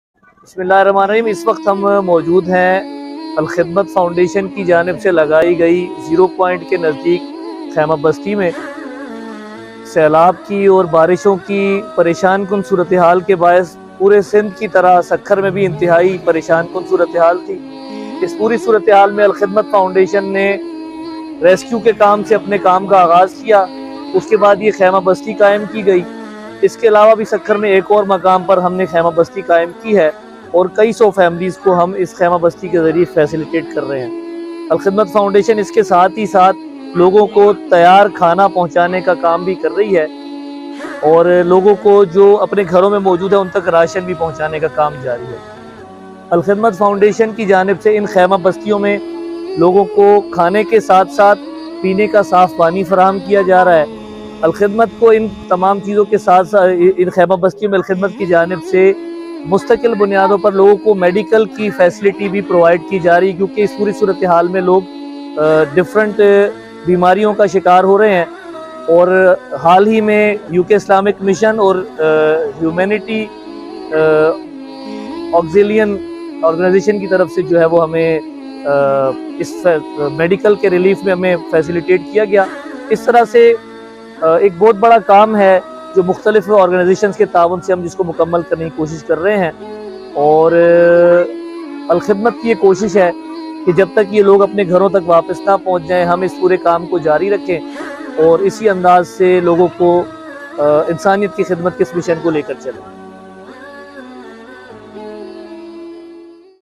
Alkhidmat Tent City Sukkur
الخدمت فاؤنڈیشن کی جانب سے سکھر کے سیلاب متاثرین کےلئے قائم کی گئی خیمہ بستی میں متاثرین کو کھانا، صاف پانی ،تعلیم اور طبی سہولیات فراہم کی جارہی ہیں۔ آپ بھی آگے بڑھیے اور الخدمت کا ساتھ دیجئے۔